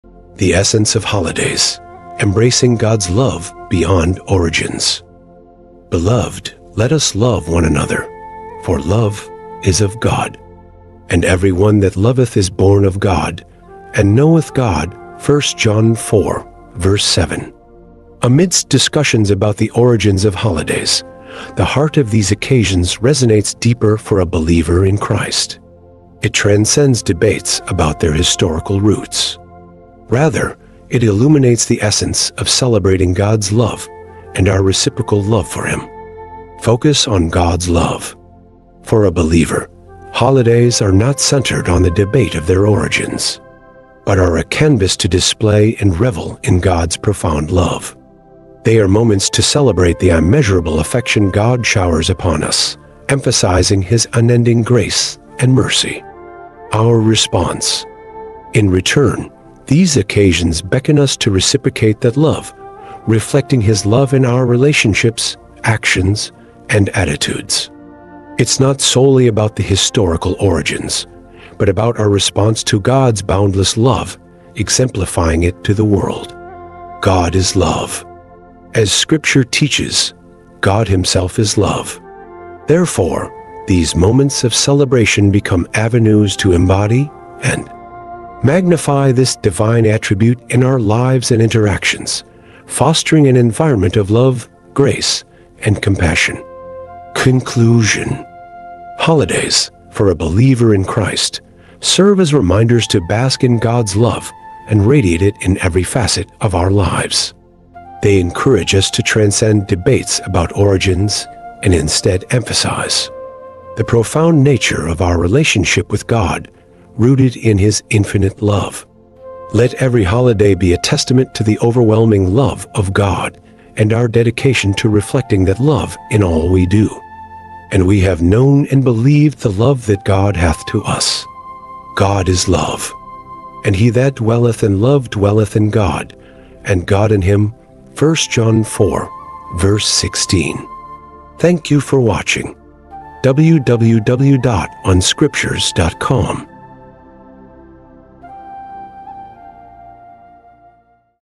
The Essence of Holidays: Embracing God's Love Beyond Origins
*"Beloved, let us love one another: for love is of God; and every one that loveth is born of God, and knoweth God."* - 1 John 4:7 (KJV)

Amidst discussions about the origins of holidays, the heart of these occasions resonates deeper for a believer in Christ. It transcends debates about their historical roots; rather, it illuminates the essence of celebrating God's love and our reciprocal love for Him.

Focus on God's Love:
For a believer, holidays are not centered on the debate of their origins but are a canvas to display and revel in God's profound love. They are moments to celebrate the immeasurable affection God showers upon us, emphasizing His unending grace and mercy.

Our Response:
In return, these occasions beckon us to reciprocate that love, reflecting His love in our relationships, actions, and attitudes. It's not solely about the historical origins, but about our response to God's boundless love—exemplifying it to the world.

God Is Love:
As Scripture teaches, God Himself is love. Therefore, these moments of celebration become avenues to embody and magnify this divine attribute in our lives and interactions, fostering an environment of love, grace, and compassion.

Conclusion:
Holidays, for a believer in Christ, serve as reminders to bask in God's love and radiate it in every facet of our lives. They encourage us to transcend debates about origins and instead emphasize the profound nature of our relationship with God, rooted in His infinite love. Let every holiday be a testament to the overwhelming love of God and our dedication to reflecting that love in all we do.

*"And we have known and believed the love that God hath to us. God is love; and he that dwelleth in love dwelleth in God, and God in him."* - 1 John 4:16 (KJV)